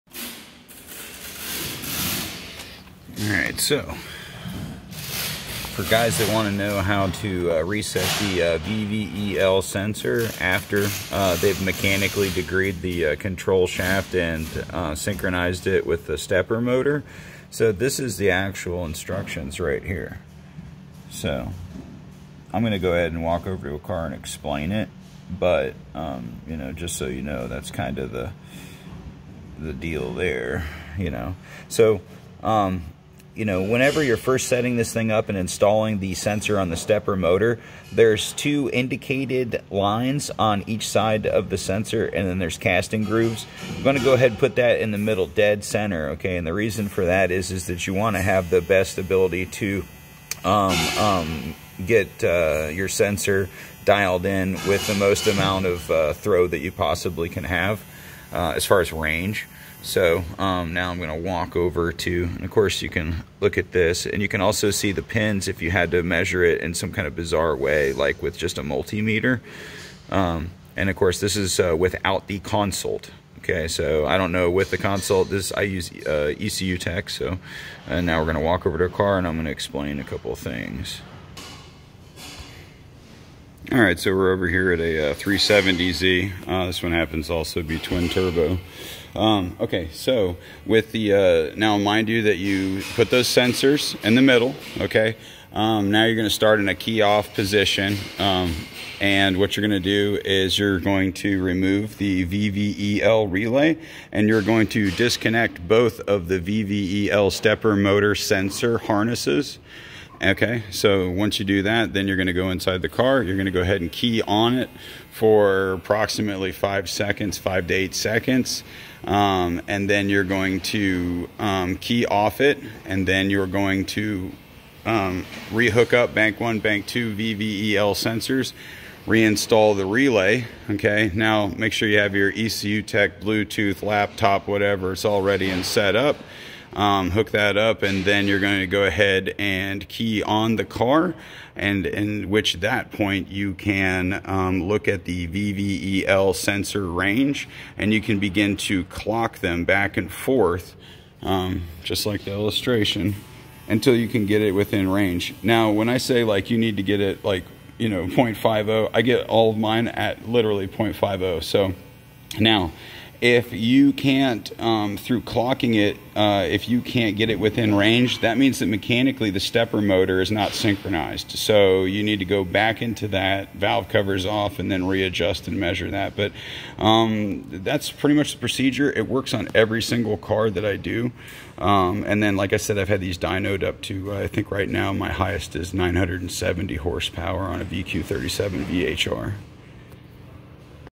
370Z/ G37:How to reset VVEL
the process of putting the vvel control module into adjustment mode